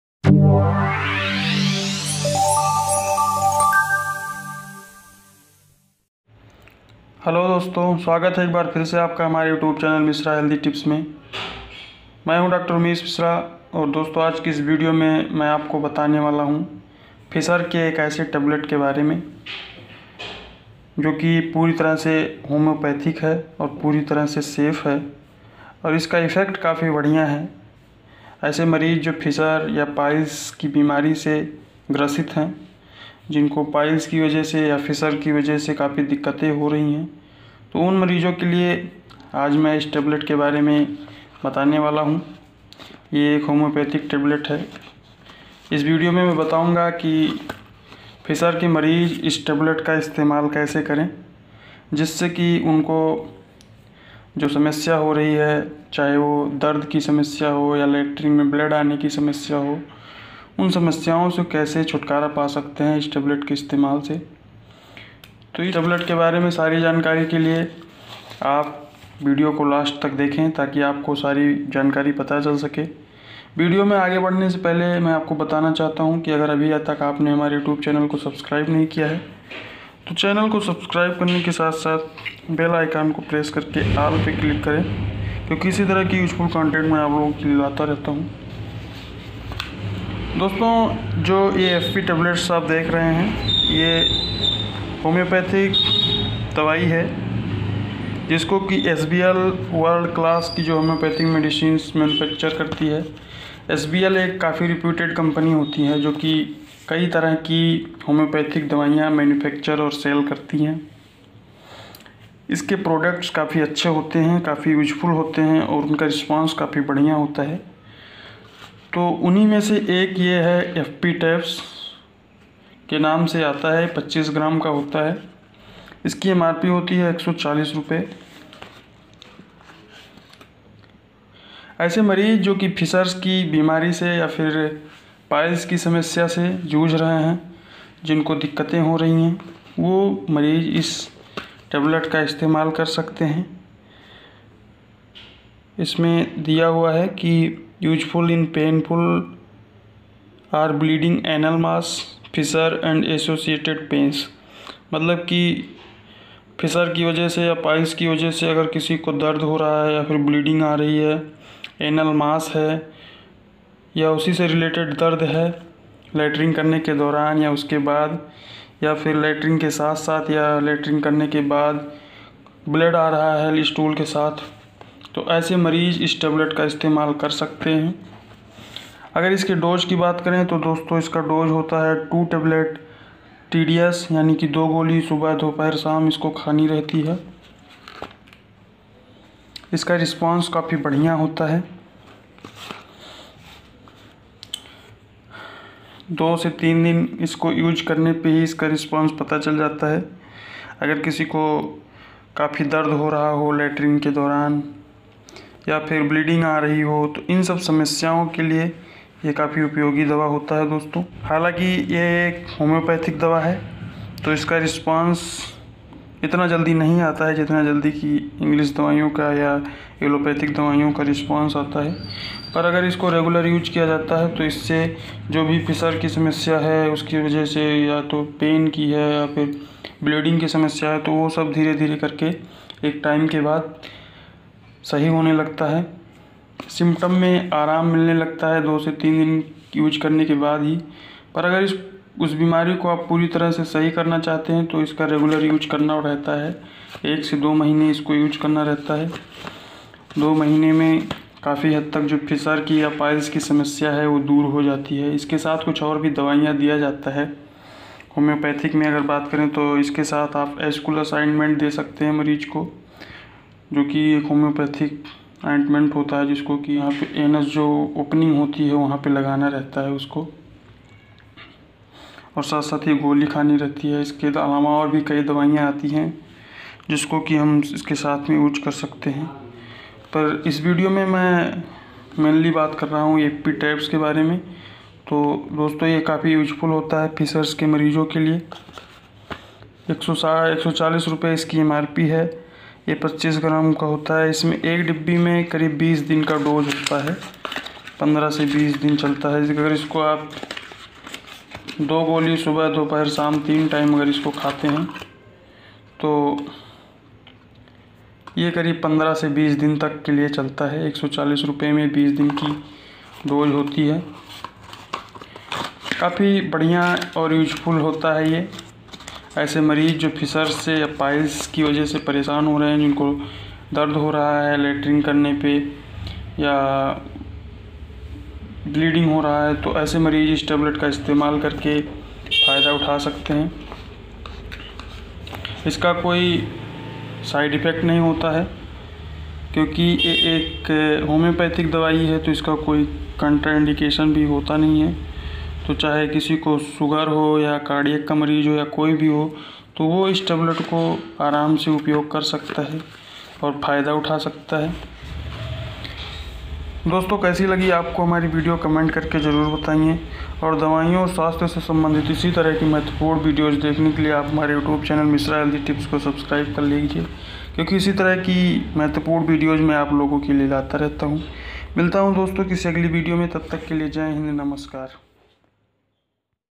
FP-tabs homopathic medicine।fissure,piles best homopathic medicine all detail in hindi by Dr umesh
Hlo friends wlcom to our channel
mishra healthy tips ।
myself Dr umesh mishra

Dosto es vedio me hum janenege
FP tabs ke baare me puri jankari
FP tabs ak homopathic medicine hai
jisko ki SBL pharma company manufacture karta hai aur distribute karta hai ।
es tablet ka upyog fissure aur piles ke patient me kiya jata hai eska effect bahut badhiya hota hai ।

This vedio containt information about tablet FP tabs
there uses
effect
side effect
indication
dose
and many more about this tablet ।
So watch the vedio till end to know all information about this tablet

and friends also support us to joining

MISHRA HEALTHY TIPS